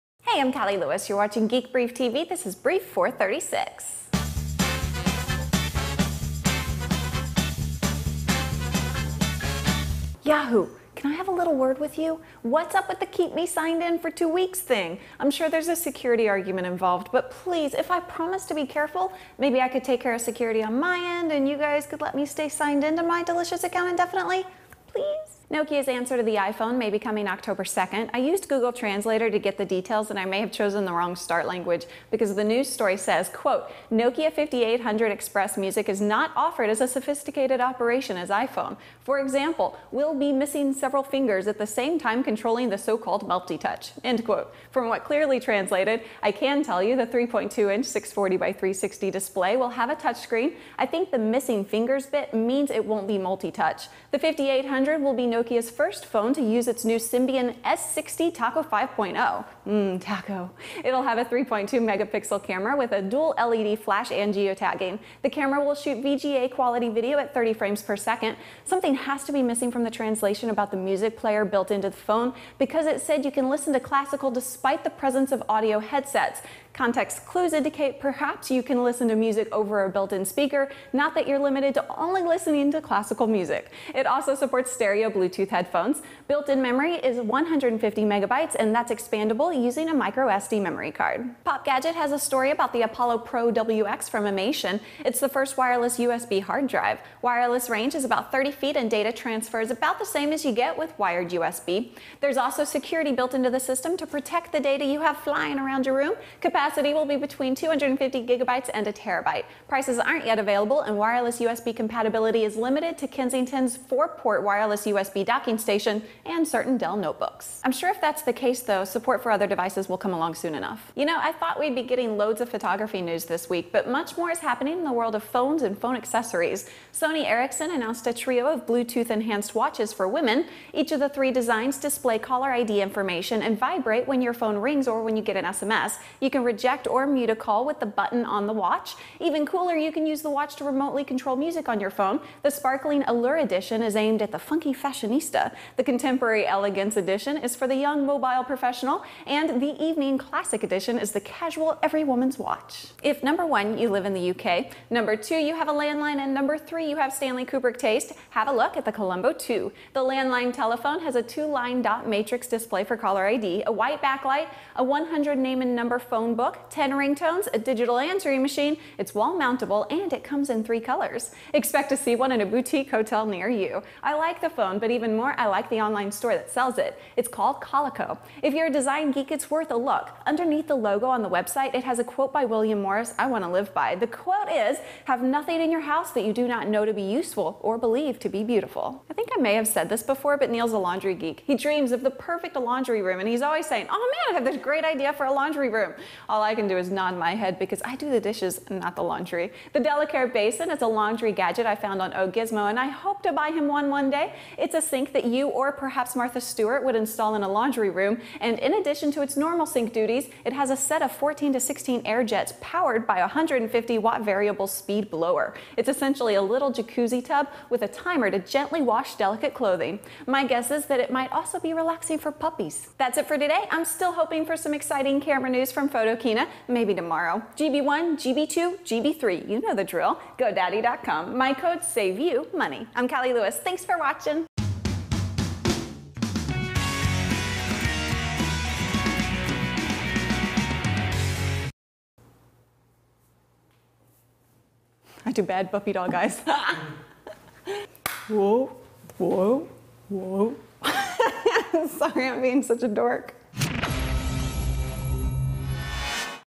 Geek Brief TV #436
The 5800 XpressMusic [1] phone might be Nokia's answer to the iPhone. The translation got a bit screwed up, but we do have some details about the phone.

  The Apollo Pro WX [2] is the first wireless USB hard drive, with capacities of 250GB to 1TB.

  Sony Ericsson announced three bluetooth enhanced watches [3] for women. Control music on your phone, see who's calling, and answer or ignore the call. They're the The Sparkling Allure Edition, The Contemporary Elegance Edition, and The Evening Classic Edition.

  The Colombo Two [4] is a landline telephone with a 2 line dot matrix display for Caller ID, a digital answering machine and more.

  The Delicair Basin [5] is for laundry geeks like Neal. It has 14 to 16 air jets in the sink for washing delicate clothing.